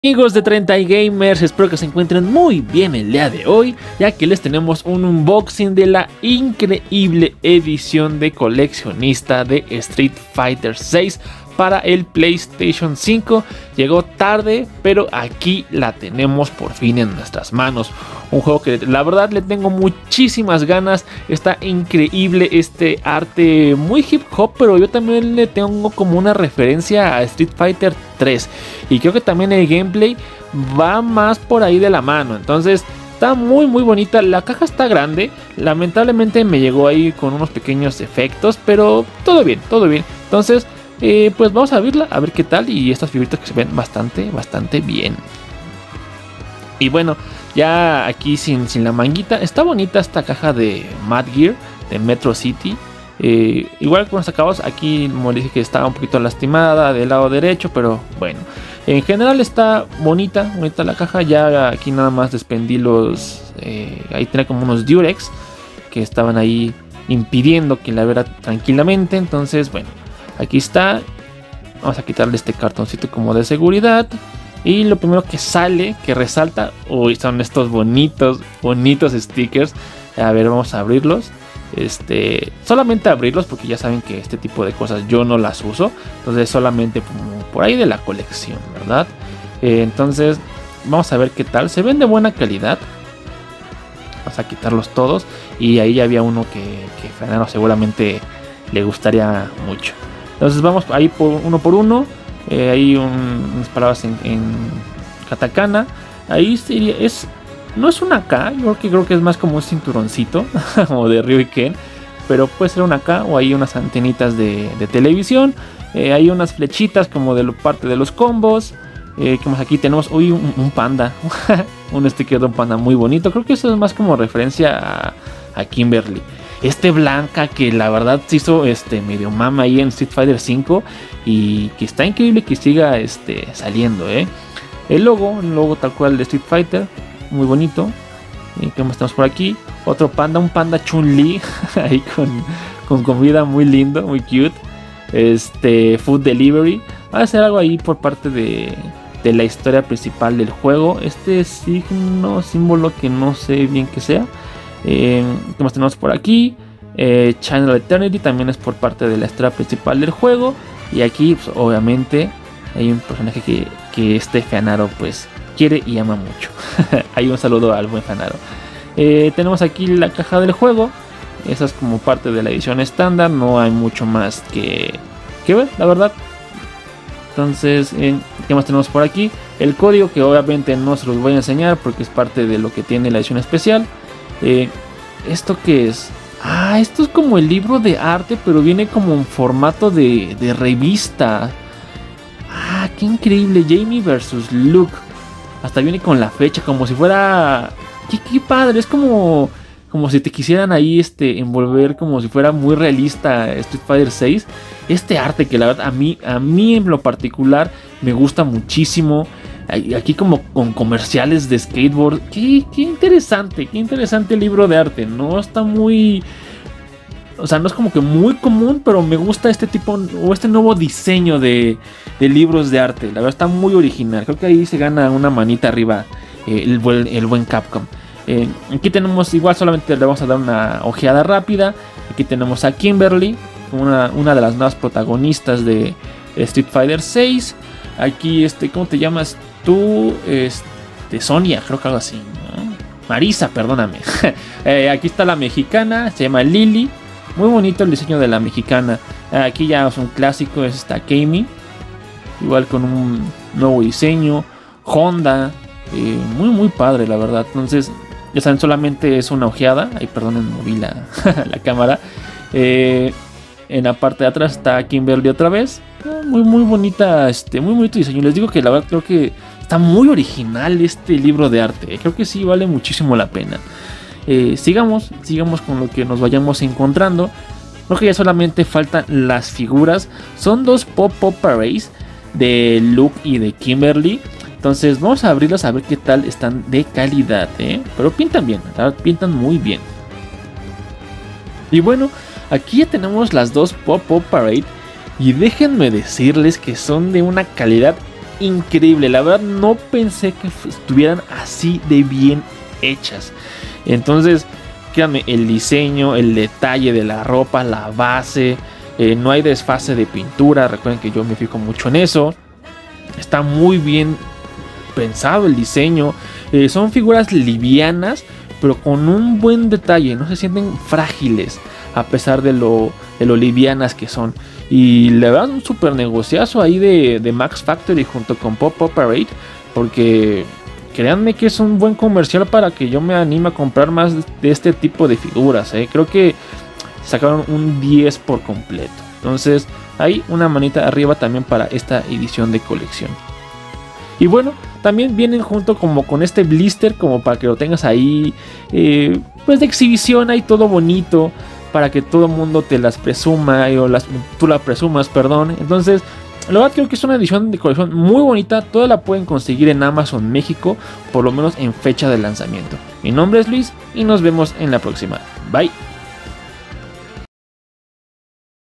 Unboxing | Street Fighter 6 Mad Gear Box | Edición de colección | Treintaigamers | 4k
En esta ocasión les traemos este Unboxing de la edición de colección de street fighter 6!, el más reciente juego de peleas de capcom y el nuevo contendiente a superar el legado de la mejor franquicia de peleas.